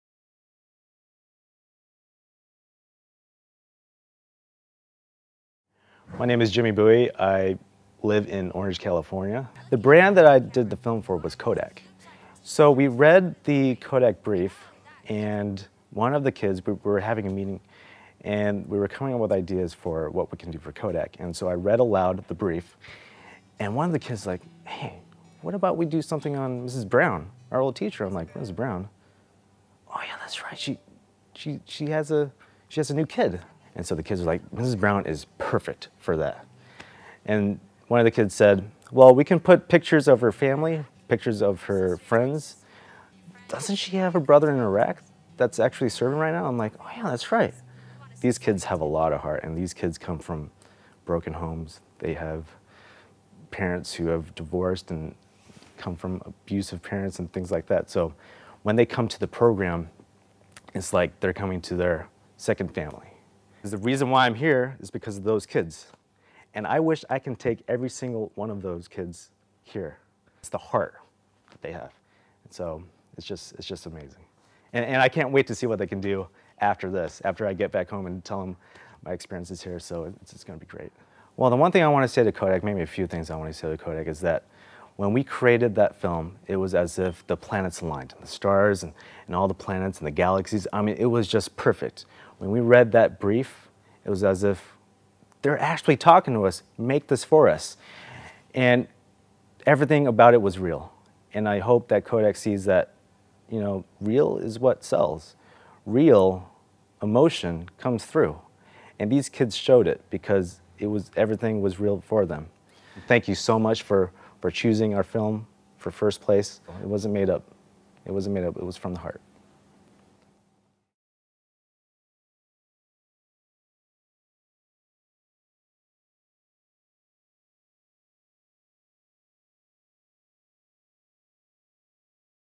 Mofilm Interview with Jimmy Bui for Kodak
This is an interview with competition winner Jimmy Bui who won the MOFILM competition  for KODAK.